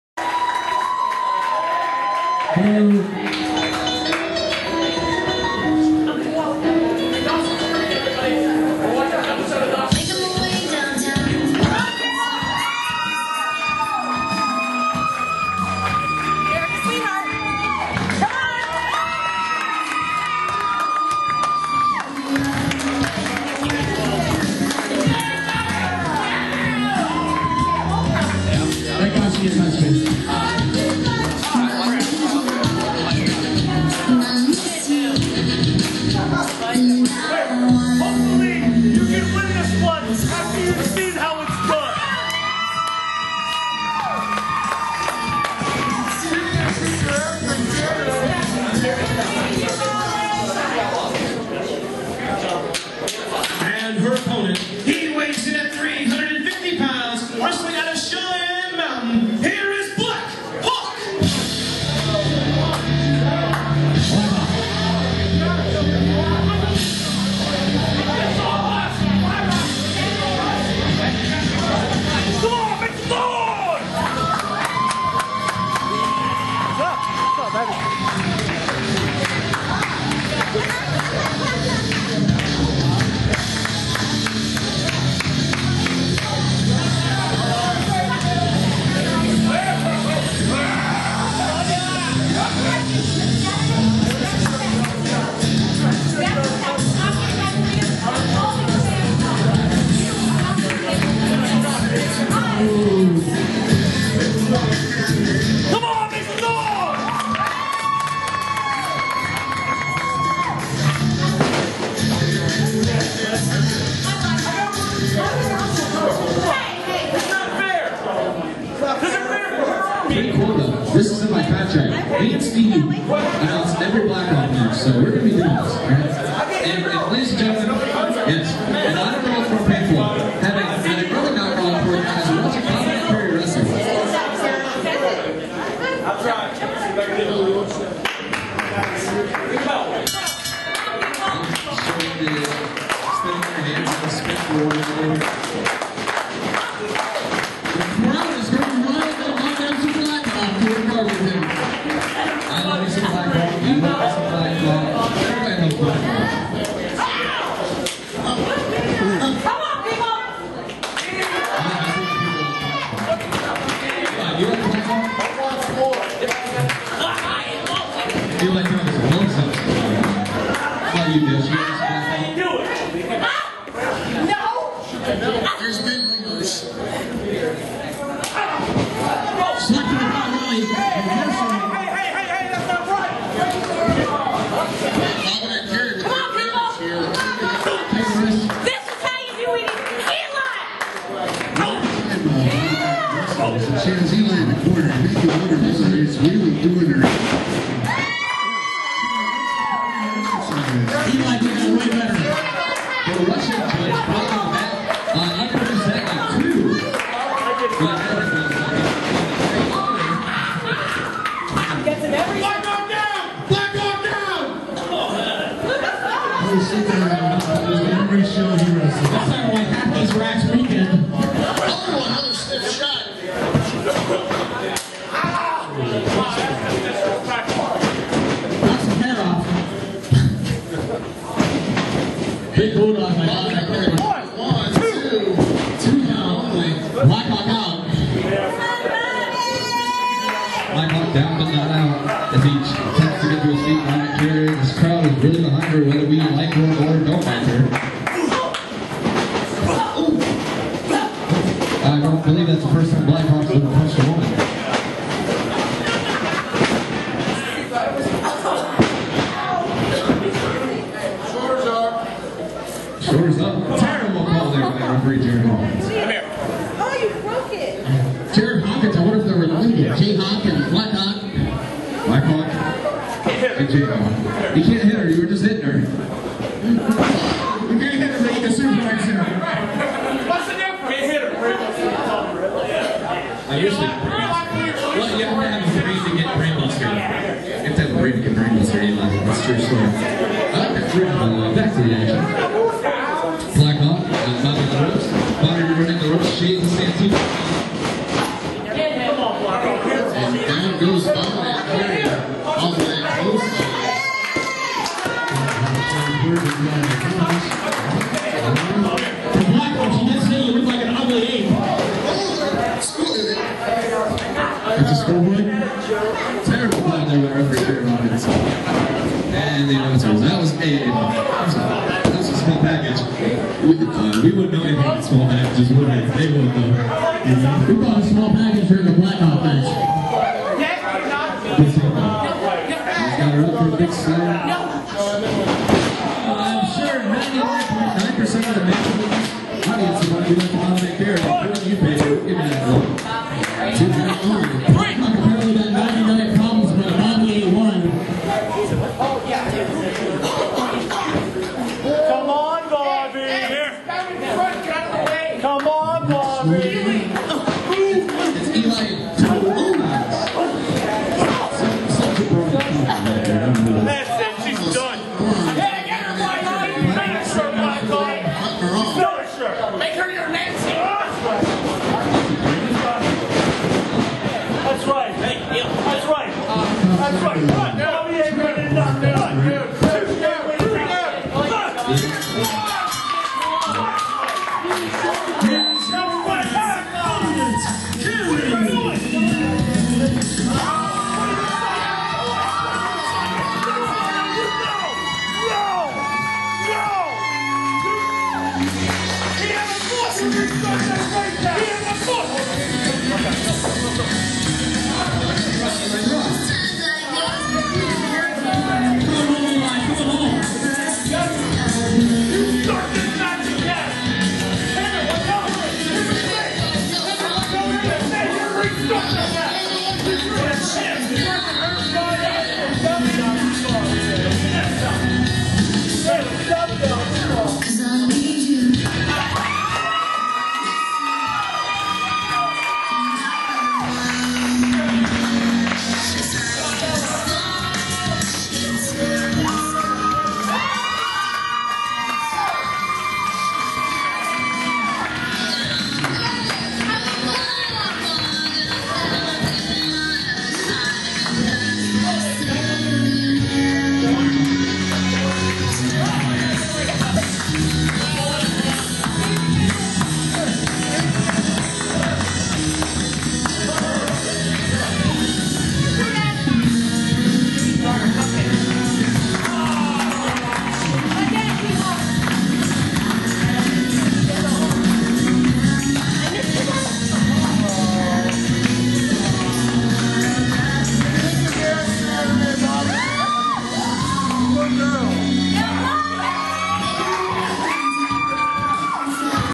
Bobbinette Carey vs Blackhawk
Heading into Saturday's show Bobbinette carey takes on the giant black Hawk